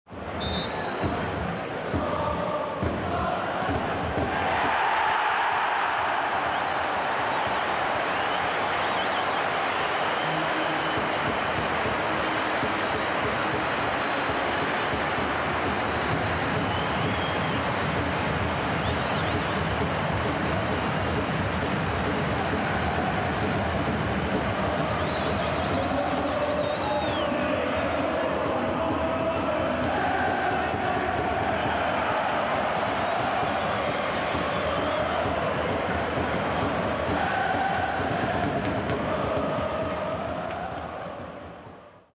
Sinclair scores winner from slick free kick move
Watch Celtic forward Scott Sinclair score his 11th goal of the season to give his side a 1-0 win over Partick Thistle at Celtic Park in the SPFL Ladbrokes Premiership.

Celtic 1-0 Partick Thistle, 20/12/2016